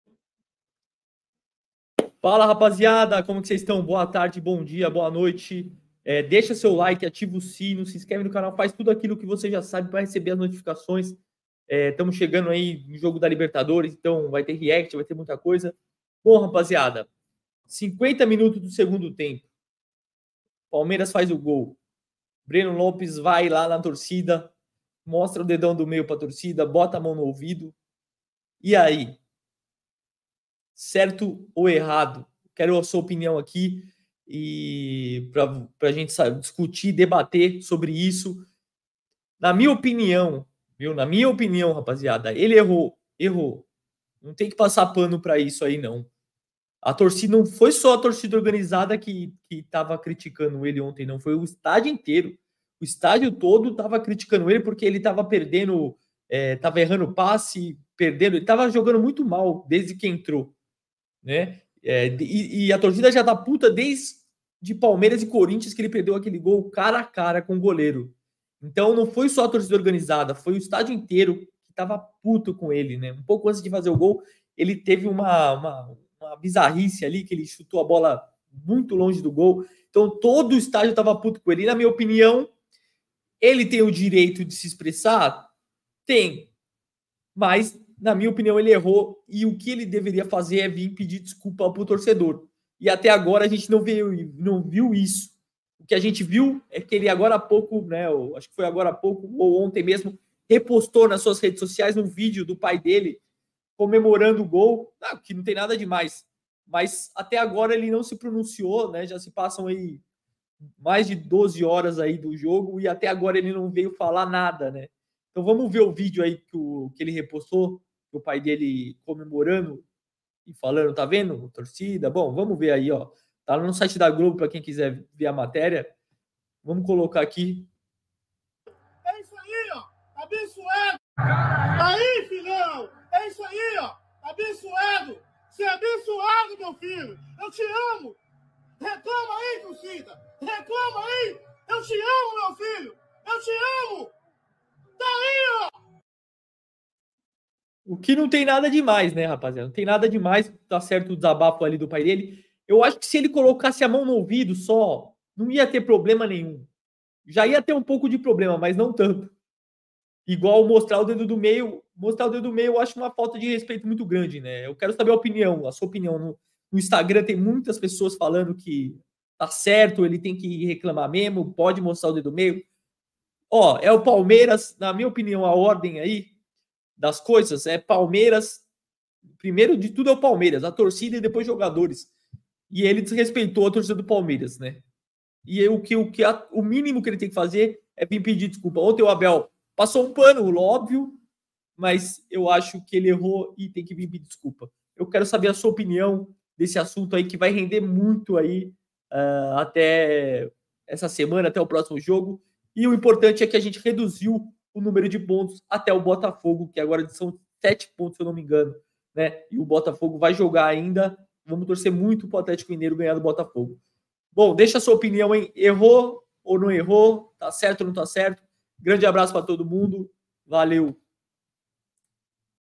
NÃO VAI PEDIR DESCULPAS? BRENO LOPES REPOSTA VIDEO EM REDE SOCIAL!
BRENO LOPES REPOSTA VIDEO EM REDE SOCIAL! NAO ESTA ARREPENDIDO?
BRENO LOPES MOSTRA DEDO DO MEIO
BRENO LOPES PALMEIRAS
Seja membro deste canal e ganhe benefícios: